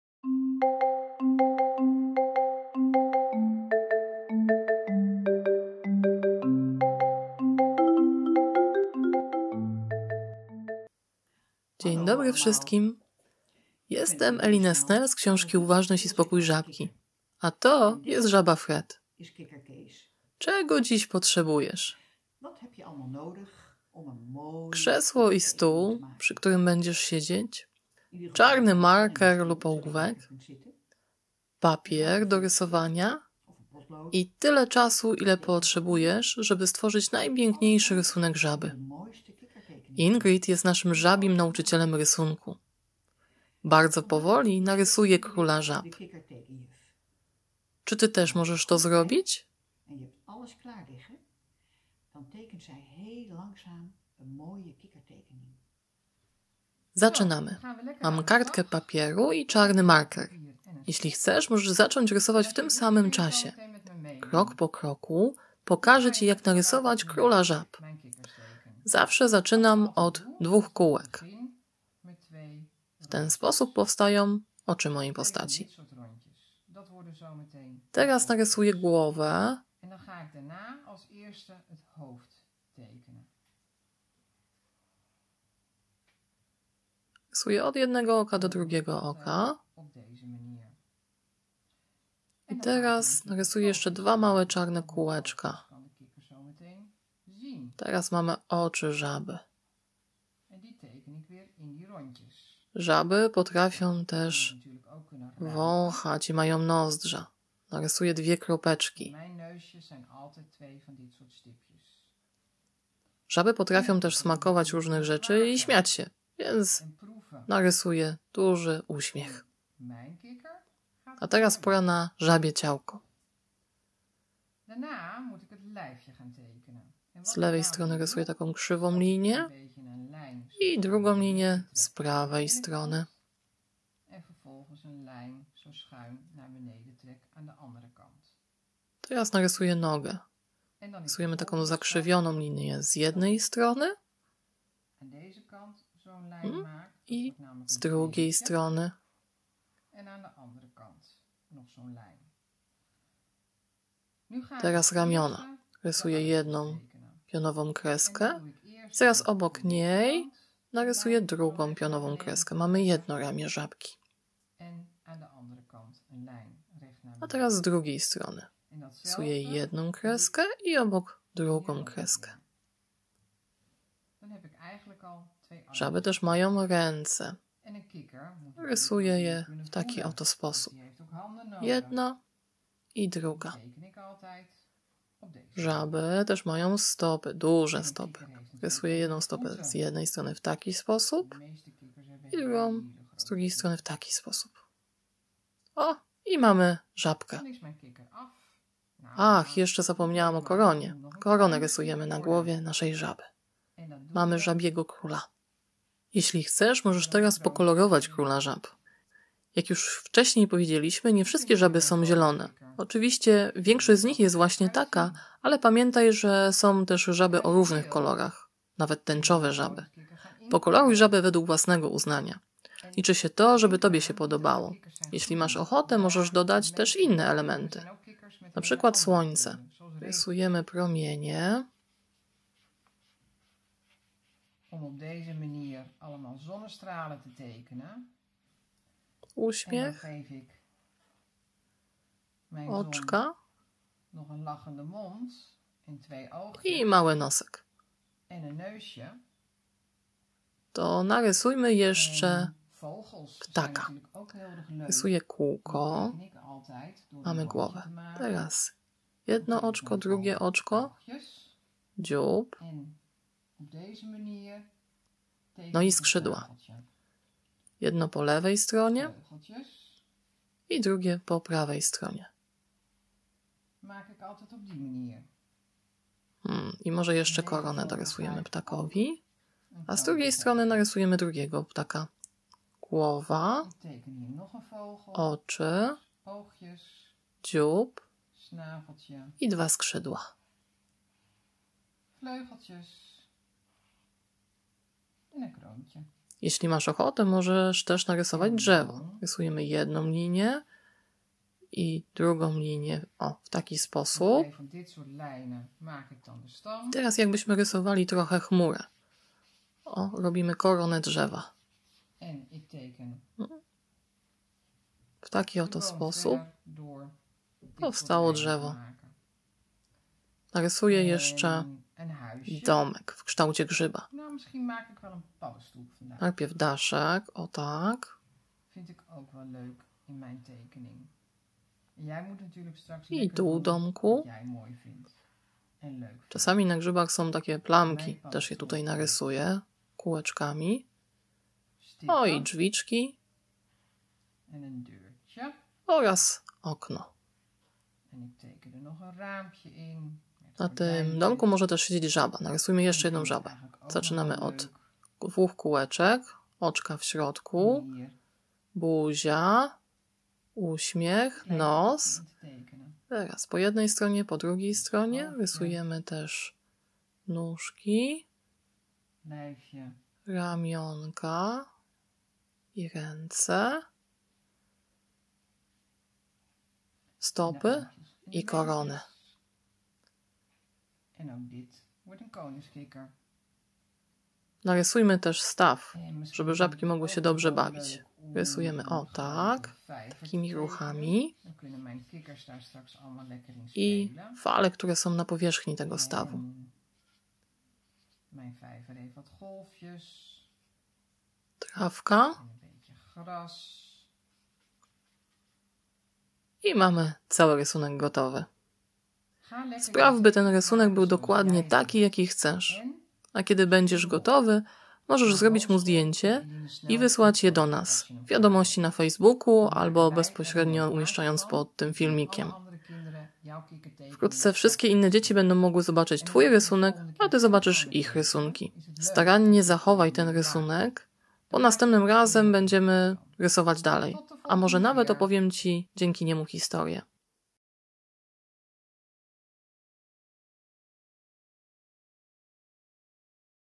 Domowa szkoła z żabą Fredem cz. 2
Domowa szkoła mindfulness dla dzieci oraz rodziców Eline Snel - autorki książek "Uważność i spokój żabki" i "Moja supermoc".

Głosu użyczają Beata Kamińska-Suchanek i Magda Kasprzyk.
Specjalnie dla żabek, które z powodu zamkniętych przedszkoli i szkół są teraz w domach.

Żaba Fred pomoże Wam nauczyć się oddechu, wyciszenia i koncentracji, opowie historyjki, zaśpiewa z Wami piosenki, porysuje... Bawcie razem się dobrze!

Kolejne lekcje będą publikowane w poniedziałki i czwartki o godz. 17.00. Do zobaczenia!